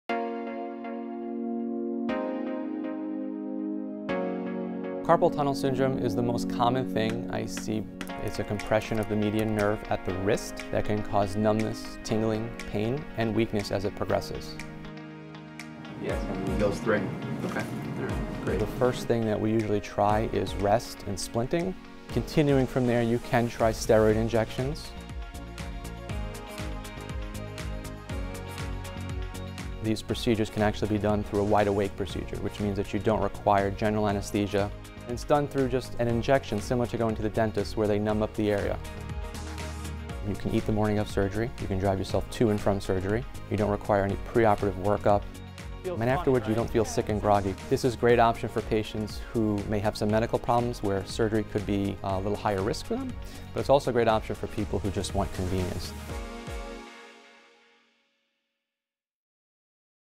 UConn Health Minute: Carpal Tunnel Syndrome
Carpal tunnel syndrome is a painful condition that affects millions of Americans. If rest and splinting don’t relieve the pain, then “wide-awake” surgery performed without sedation may be an effective and convenient option.